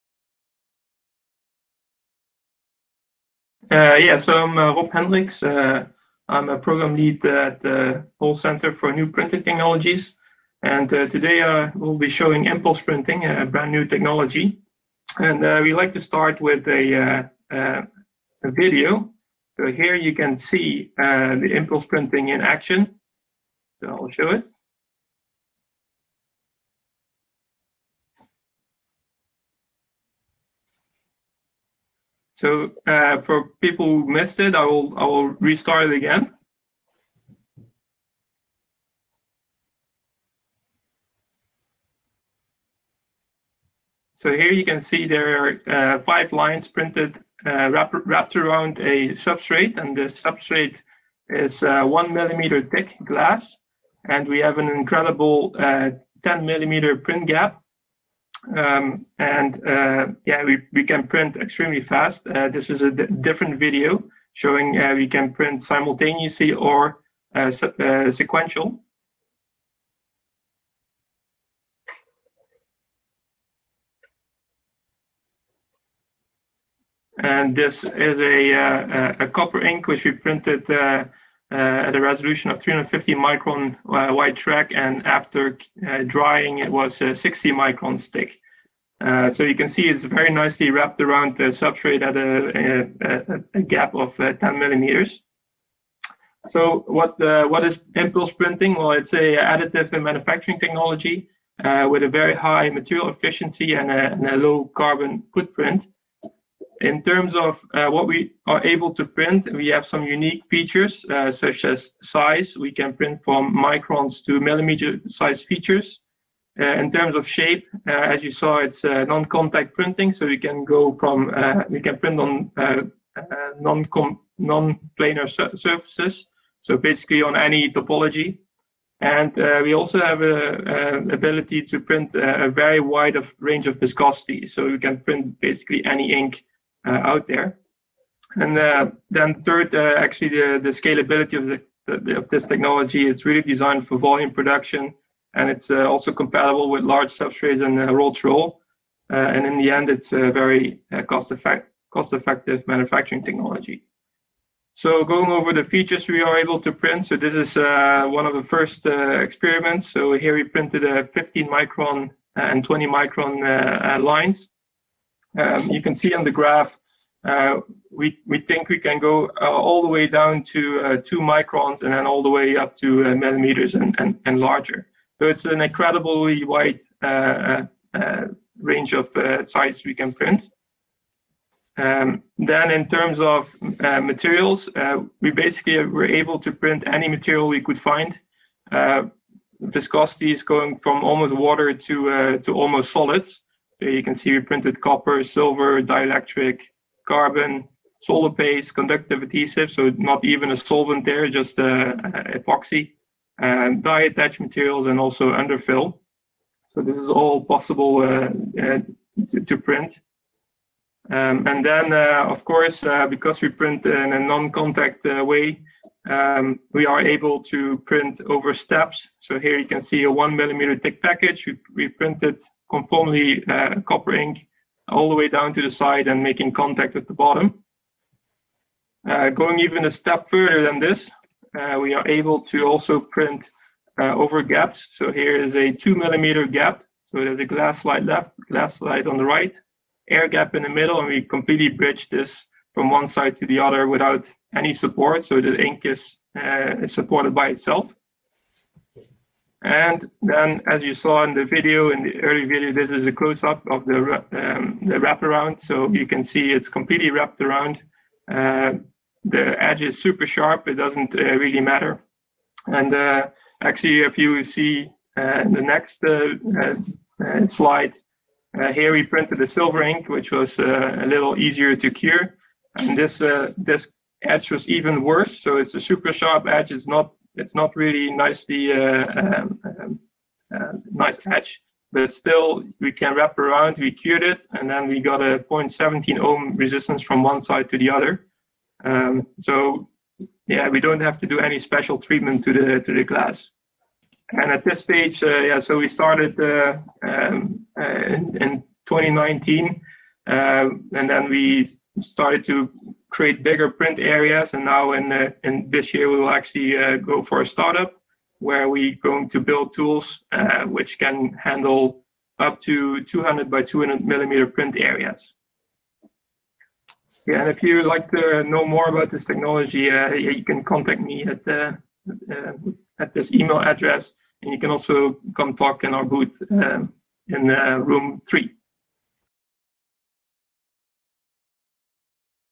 TNO@Holst Centre | Impulse Printing™
Speaker:
Rob Hendriks, Program Manager – High-resolution Printing, TNO Holst Centre